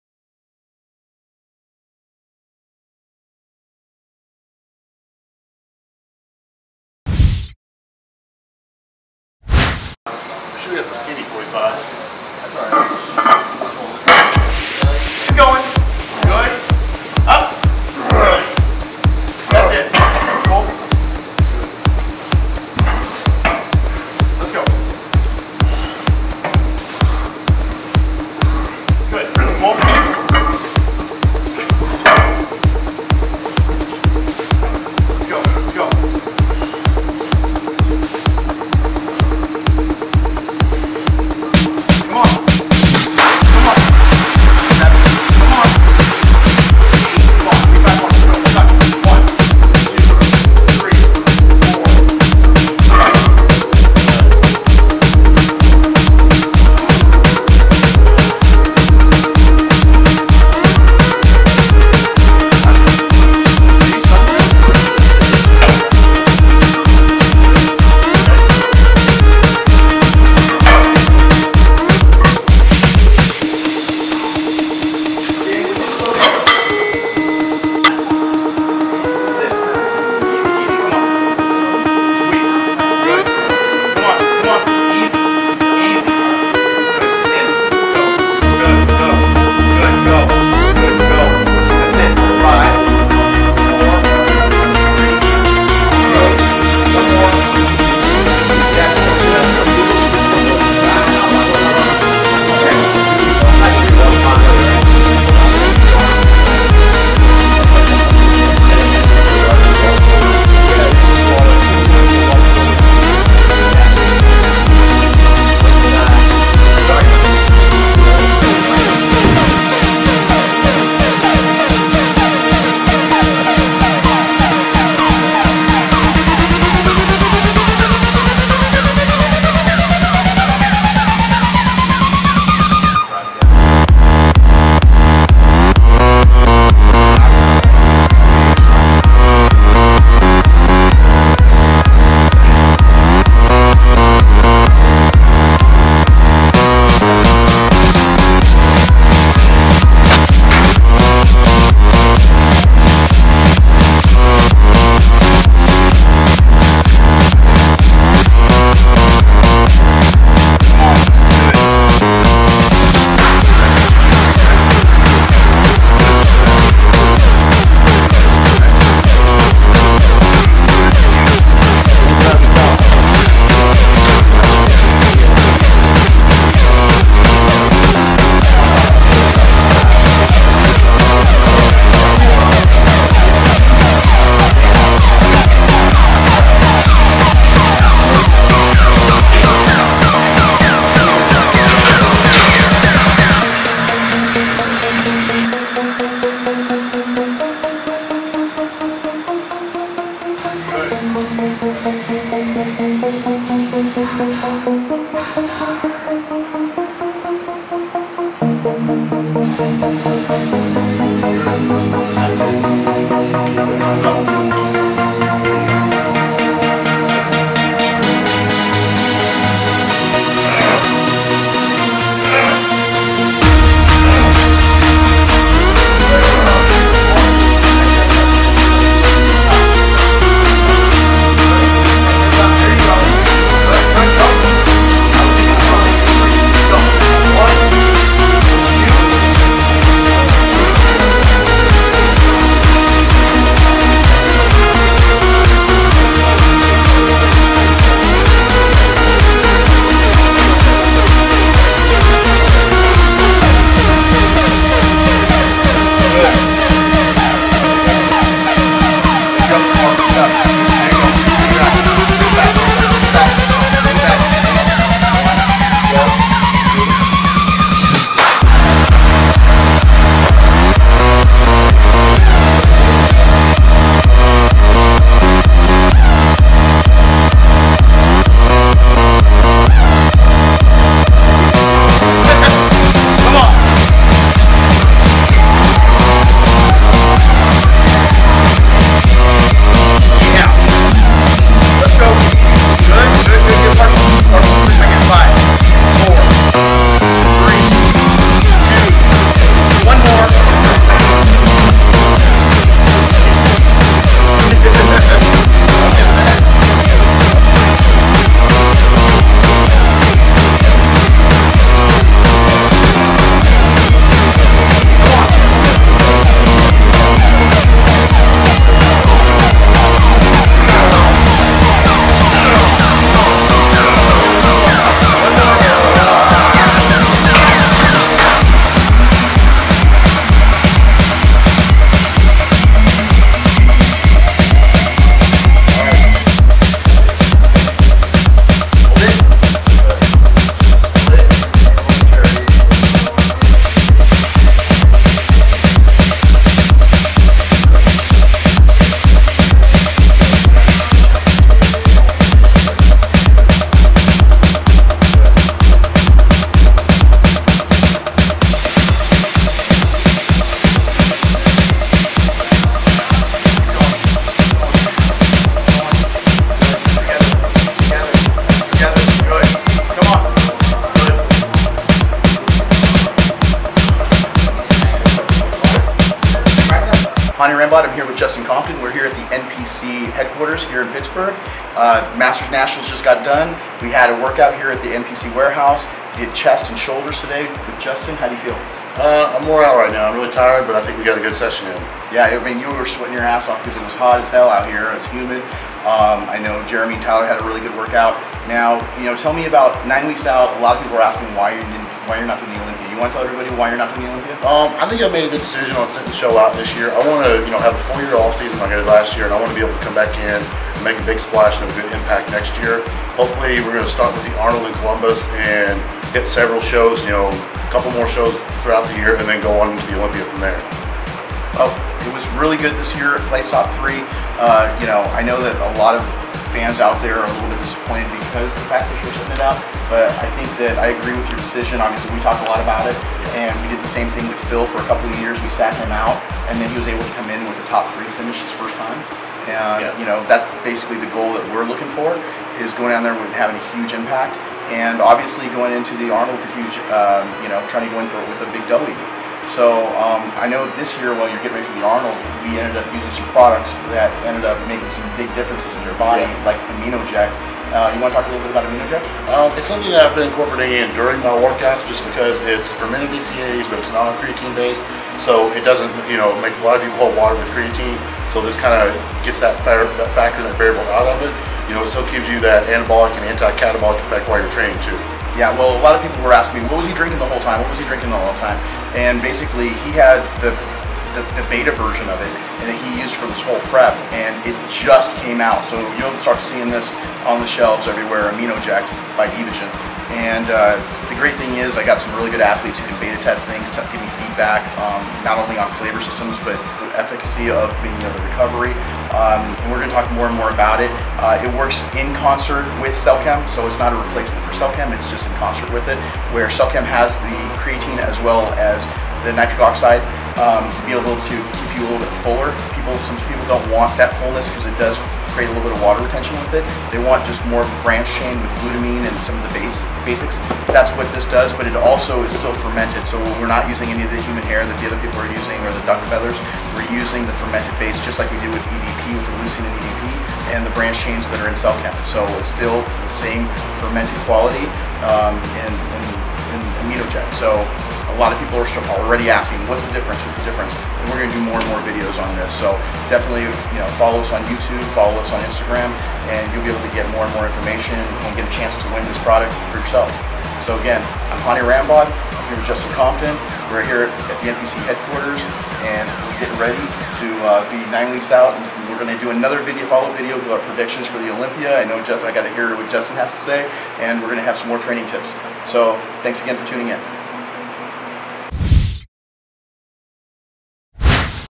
IFBB Pro Justin Compton Training Chest and Shoulders at the NPC Photo Gym
IFBB Pro Justin Compton Training Chest and Shoulders with Hany Rambod at the NPC Photo Gym